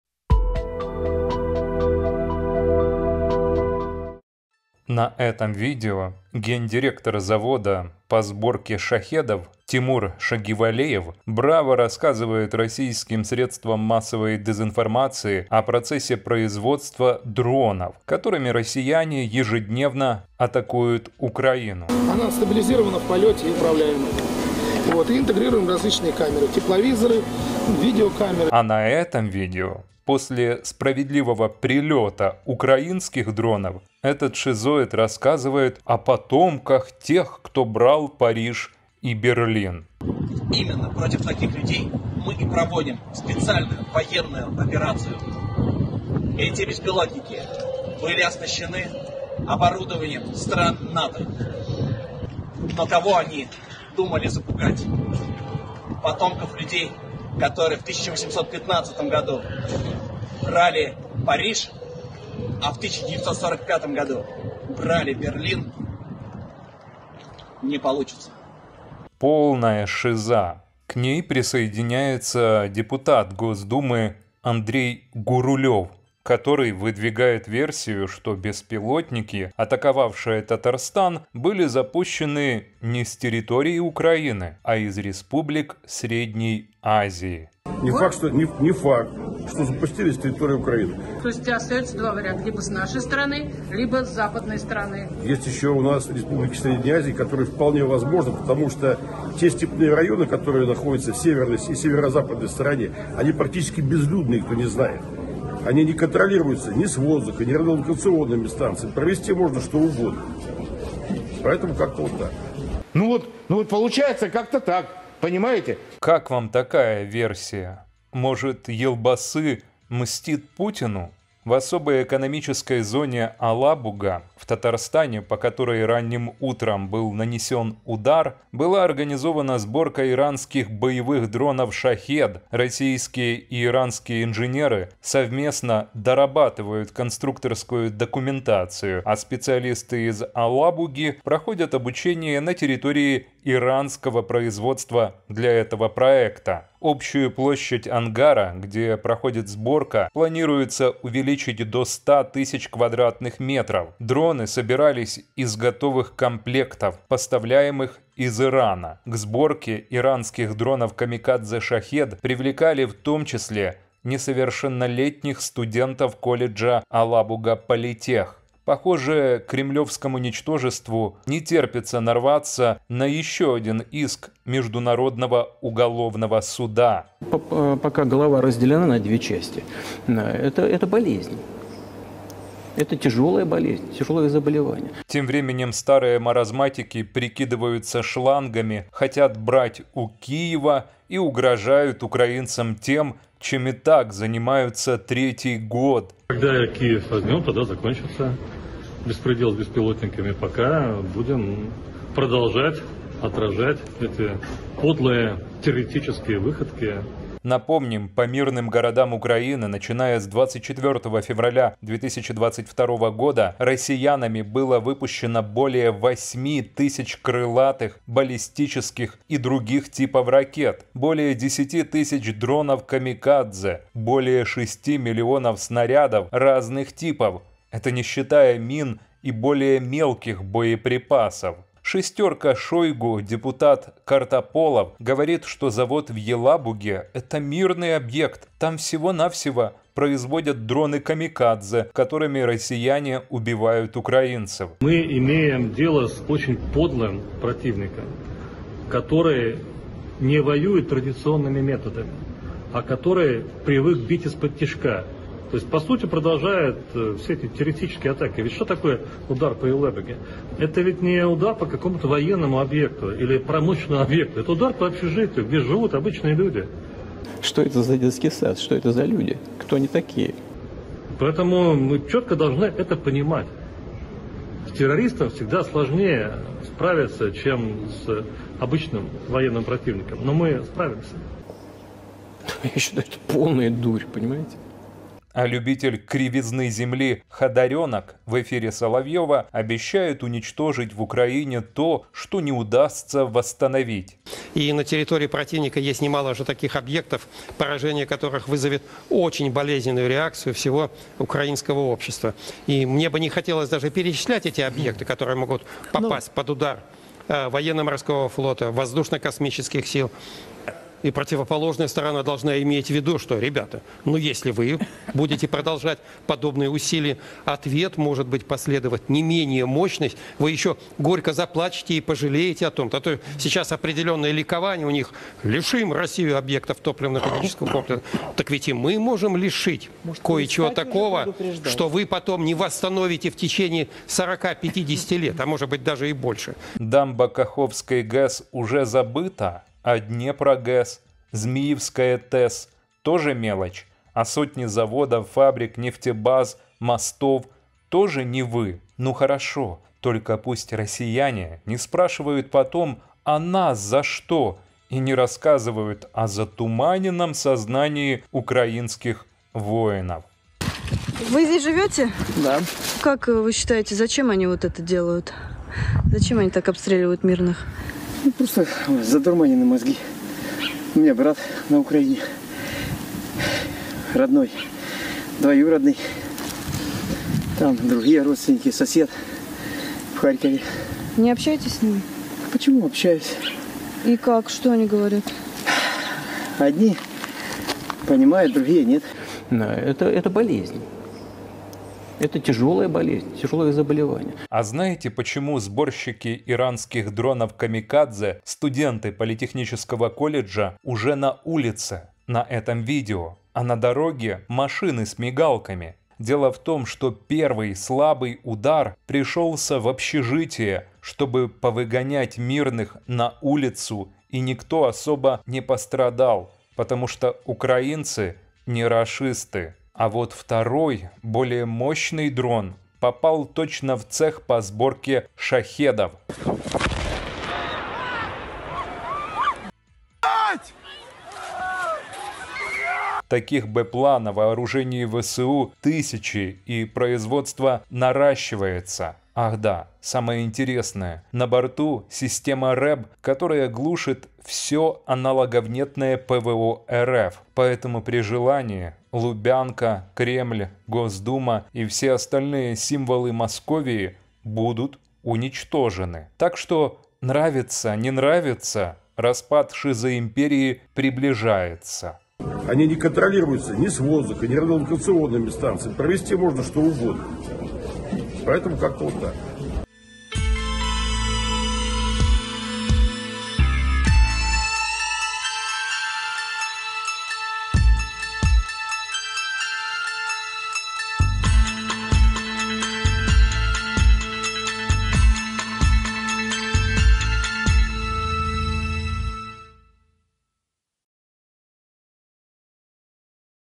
ВСУ подбираются к Уралу. Татарстан лишился НПЗ и завода Шахедов
Дроны по Татарстану могли прилететь из Казахстана. Возьмем Киев и дроны перестанут прилетать. Мы еще не начинали. Через лет 10 начнем!